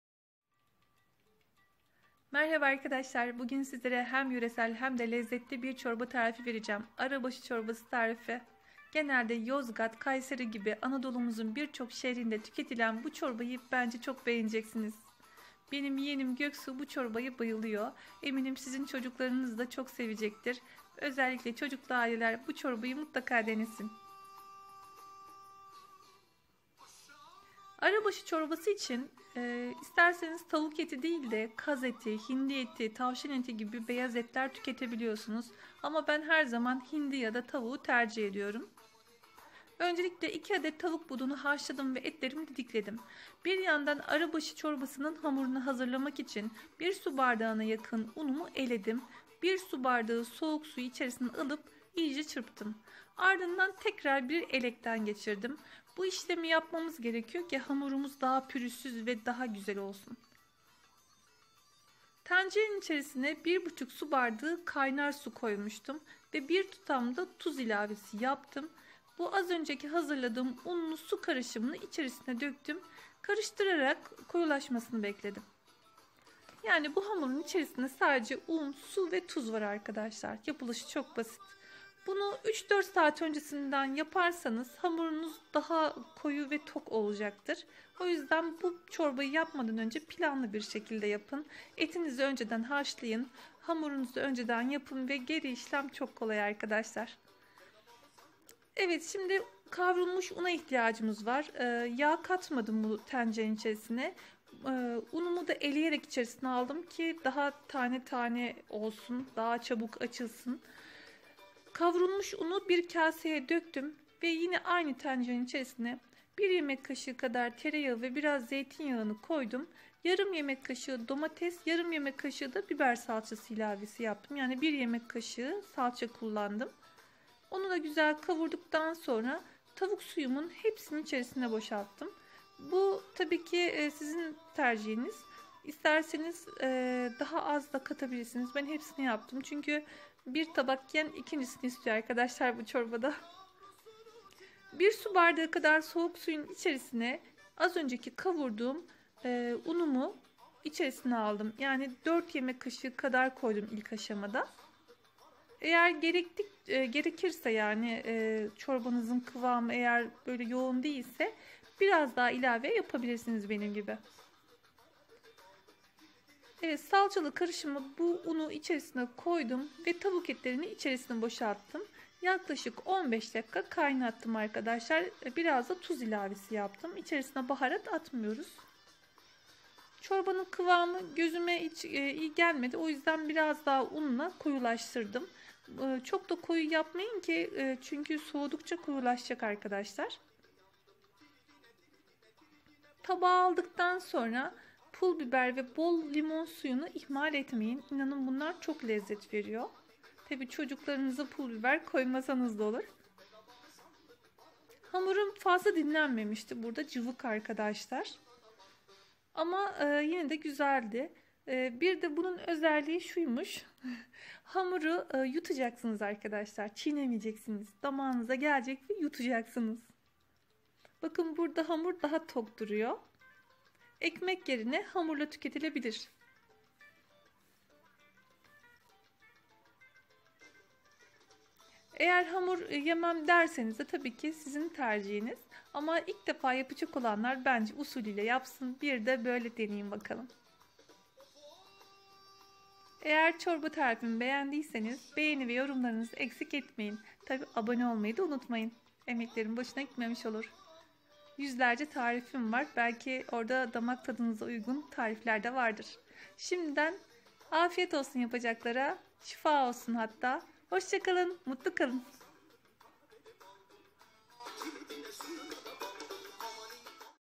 Arabaşı Çorbası ve Hamuru Nasıl Yapılır?
İç Anadolu'da kışın gelmesi ve havaların soğuması ile birlikte hemen her mutfakta pişirilen bu çorbaya bayılacaksınız Asırlardır Yozgat Kayseri Karaman Eskişehir Konya gibi illerimizde sık sık tüketilen bu çorbayı Herkesin bilmesini isterim kış mevsiminin vazgeçilmez yöresel yemeklerinden biri olan Arabaşı bu günlerde oldukça popüler hindi kaz ve tavuk gibi kanatlı hayvanların eti ile pişirilen sıcak çorba ile soğuk hamurun buluştuğu Arabaşı yemeği Anadolu'da komşularım ve akrabaların bir araya gelerek kaynaşmasına da vesile olur arabaşı çorbası un ve yağın kavrulması ile oluşturulan alzemenin tavuk suyu ile kaynatılması ile yapılan bir çorba İç Anadolu'nun birçok kentinde soğuk Kış gecelerinde bolca tüketilen çorba ekmek yerine özel yapılan hamuru ile servis ediliyor arabaşı çorbası akrabalık ve komşuluk ilişkilerinin güçlenmesine vesile oluyor derin sohbetlere eşlik ediyor oldukça acı olan tavşan tavuk veya hindi etinden yapılan çorba besin değeri yüksek gıda olarak da biliniyor

Malzemeler ve hazırlanışı
2 adet tavuk budunu bol su ve biraz tuz ile haşlamaya bırakalım tavuk etlerini didikleyelim arabaşı çorbasının hamuru için bir bardak soğuk suyun içerisine bir bardağa yakın unumuzu eleyerek alalım karıştıralım tekrar deneyelim bu şekilde daha pürüzsüz olacaktır tencerenin içerisinde bir buçuk su bardağı sıcak kaynar su bir tutam tuz üzerine bu hazırladığımız un su karışımı dökelim ve koyulaşana kadar çırpalım hamurun uzun çokluğuna göre bir tepsi seçip içerisinde ıslayalım hamuru içerisinde döküp hamuru Yaklaşık 3 saat kadar dinlendirin buzdolabında bir tencerenin içerisinde bir kase kadar un kavuralım onun kokusu çıktıktan sonra kaseye geri boşaltalım aynı tencerenin içerisine tereyağı ve zeytinyağı koyup yarım yemek kaşığı domates yarım yemek kaşığı biber salçası koyup kavuralım üzerine tavuk suyu ilave edelim kavrulmuş 4 yemek kaşığı unu biraz soğuk suyla açtırıp bu salçalı karışıma dökelim Eğer un yeterli gelmezse biraz daha un ilavesi yapabilirsiniz biraz koyu kıvamlı bir çorba olması gerekiyor suyunuza göre ayarlayın biraz tuz ilavesi yapalım tavuk etlerini koyalım Yaklaşık 15 dakika kaynamaya bırakalım daha sonra servis yaparken üzerine bol pul biber döküp biraz da limon suyuyla hamurdan parçalar kopartıp afiyet diyelim şimdiden deneyecek olanlara afiyet Şifa olsun özellikle çocuklu aileler Bence bu tarifi sık sık yapıp Kış mevsiminde çocuklarının sevdiklerinin hastalıklara karşı dirençli olmasını sağlayalım Şifa olsun kanalıma abone olursanız beğeni ve yorum yaparsanız memnun olurum